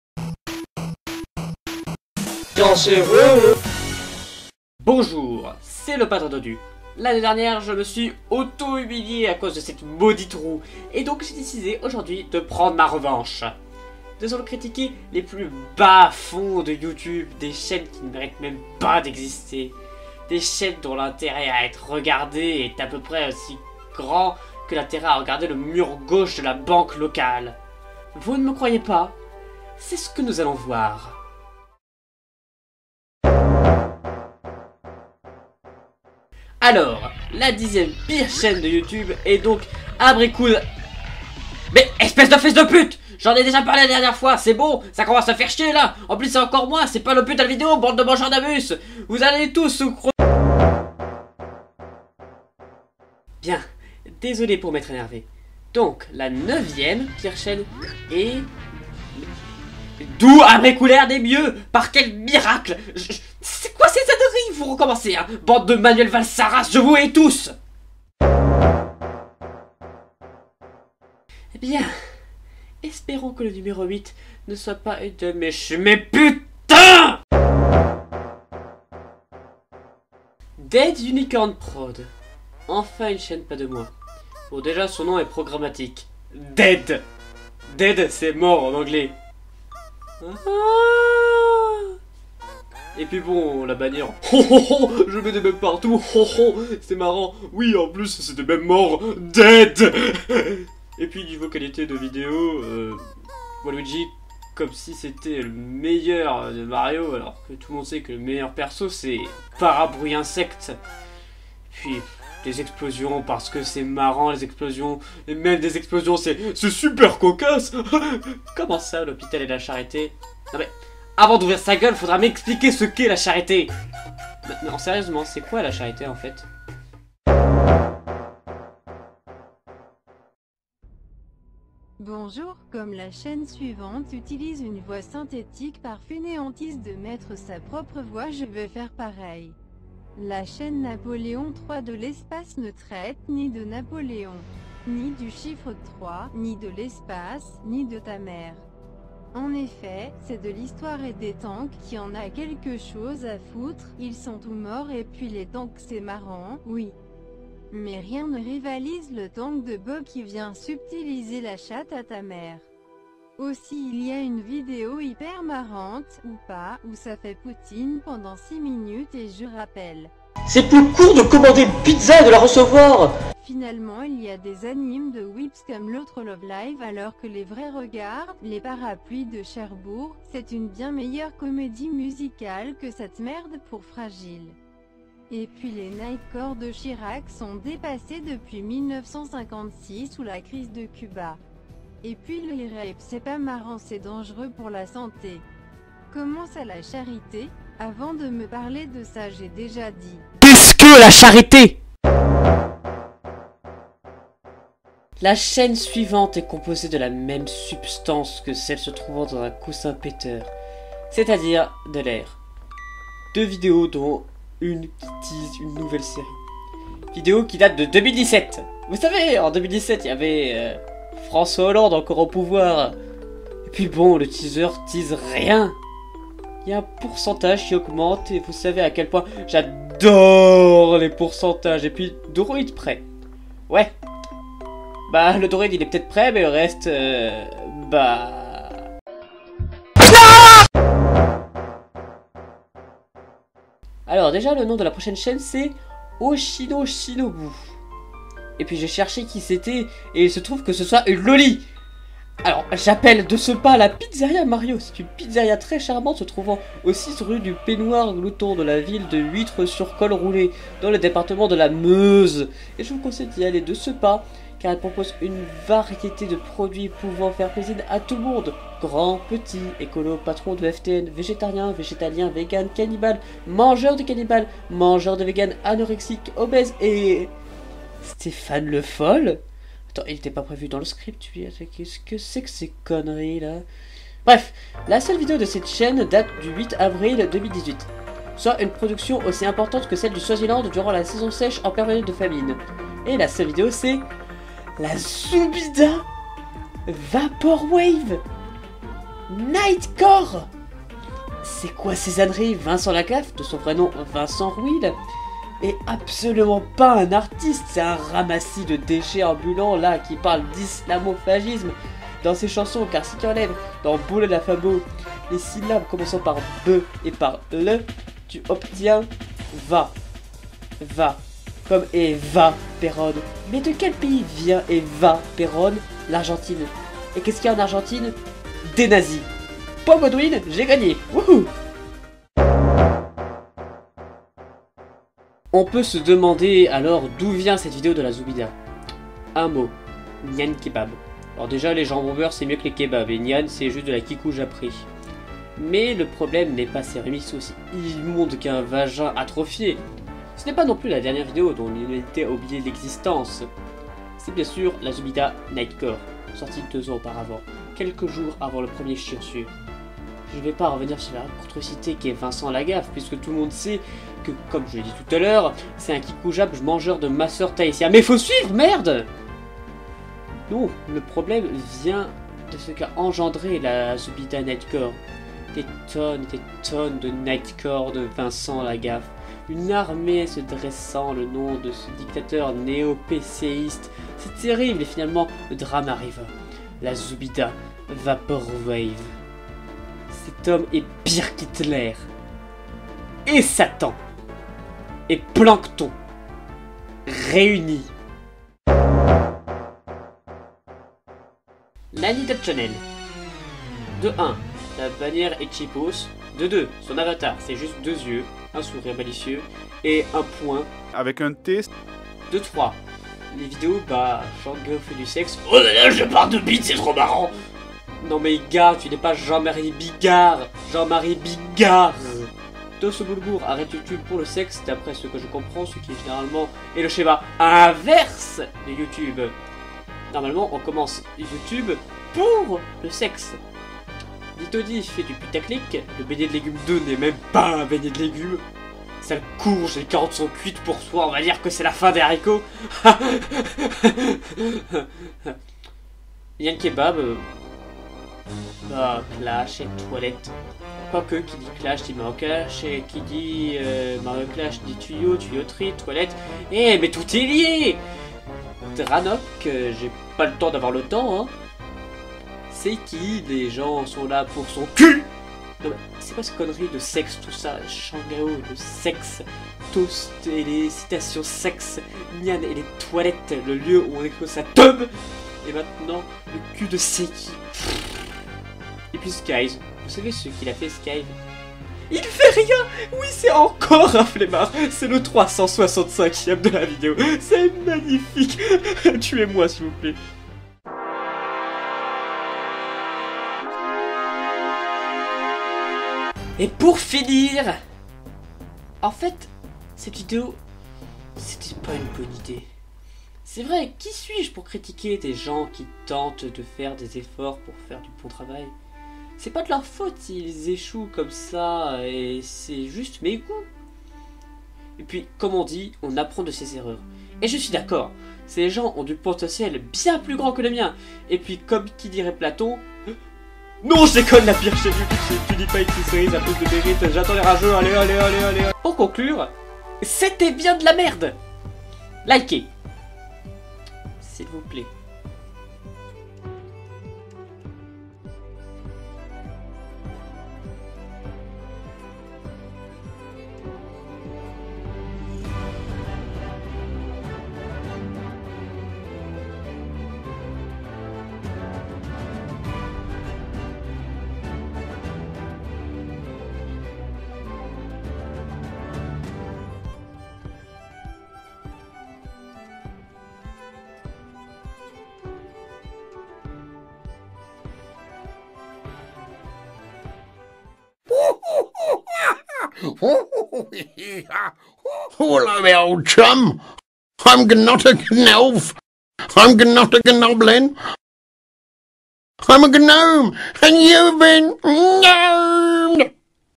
CHIONS SUR ONE SHOT - Les pires youtubeurs (objectif)
Bonjour c'est El Padre Dodu pour cet épisode spécial Premier Avril. Comme le nom de la vidéo l'indique, tout est objectif et si vous n'êtes pas d'accord vous êtes de négationnistes. Bisous.